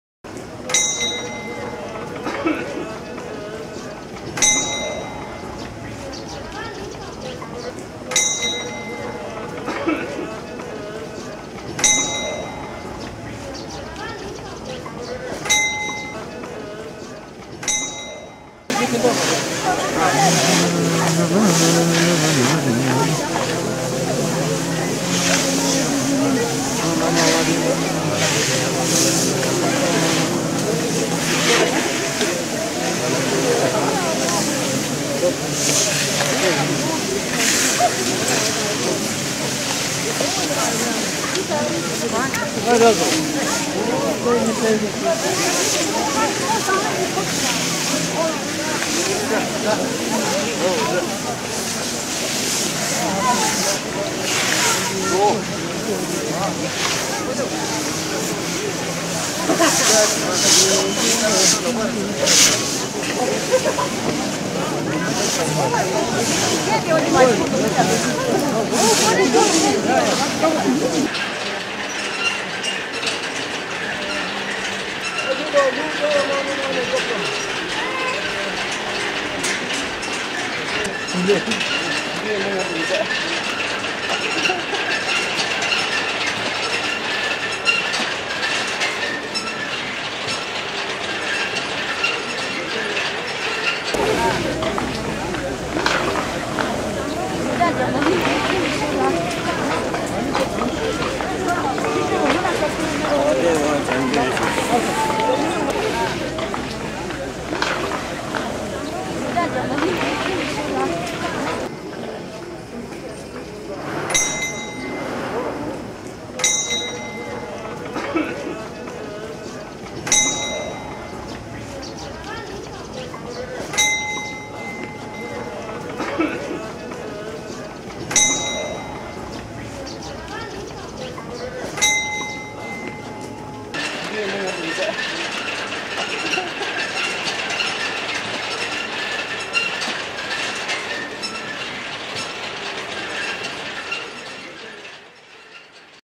Lhasa Winter Pilgrimage 拉萨市 西藏自治区
At 4000m (12.000 feet) a pretty cold place in winter. But an interesting season. Tourists left long since and town is full of Tibetans coming from all over the country for Pilgrimage.
拉萨市  西藏自治区